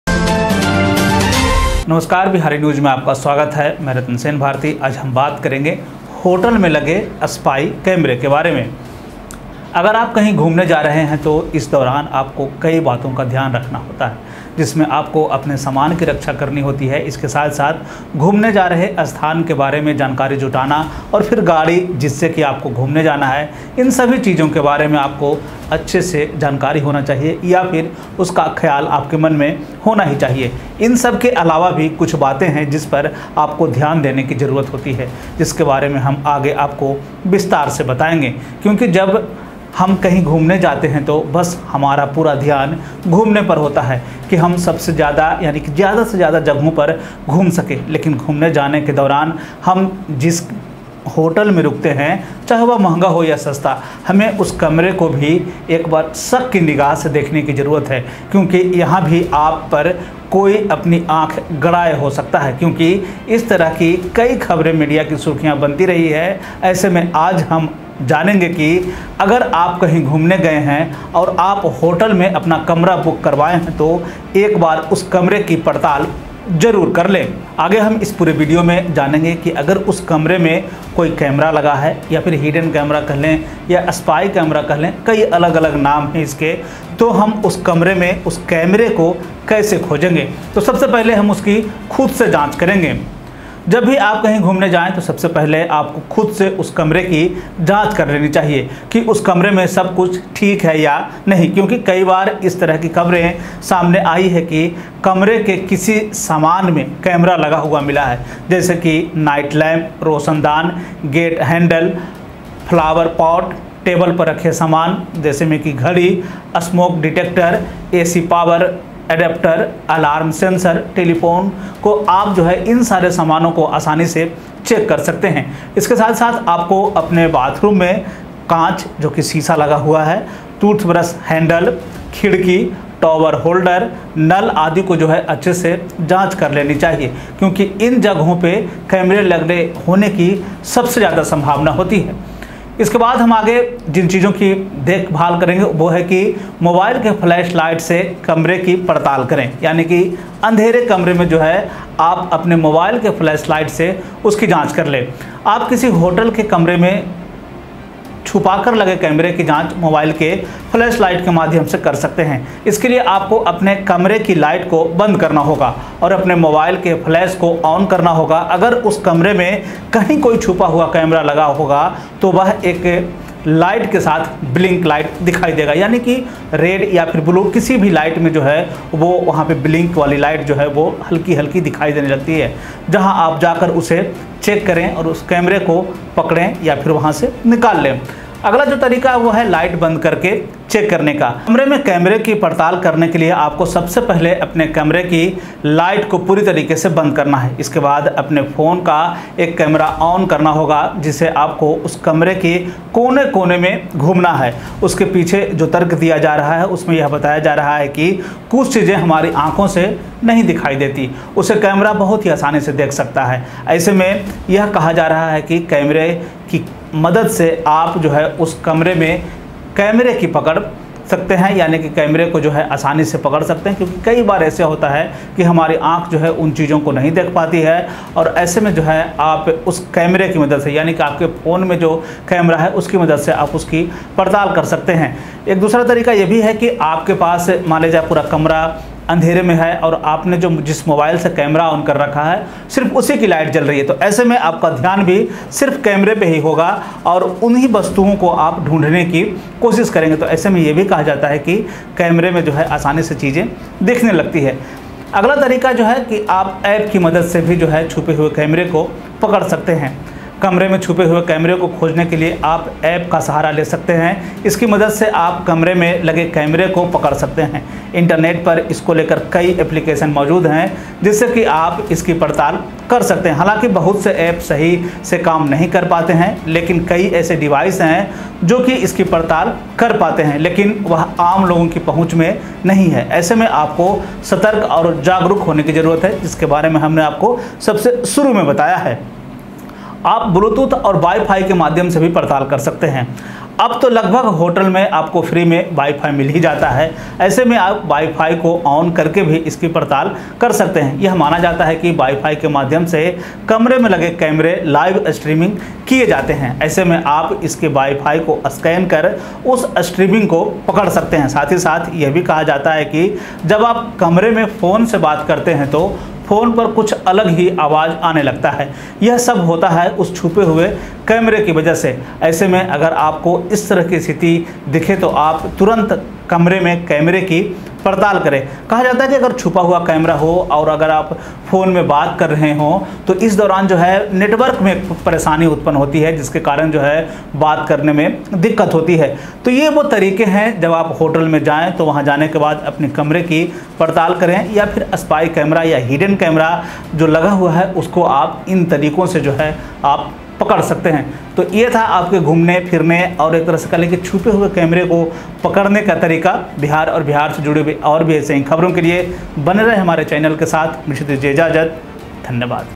इस तरीके से पकड़े होटल के कमरे में लगे कैमरे को
Whenever we go for a walk, we rent a room in the hotel. But many times such news makes media headlines in which a spy camera is installed in the hotel room. Although it is illegal, their hotel may be closed due to such acts. In such a situation, only after going to the hotel room, we need to investigate it thoroughly. Also, those places need to be checked first which they have used as decoration. Because in these there is a risk of breaching your privacy. In such a situation, you can find the hidden camera with the mobile app, WiFi Bluetooth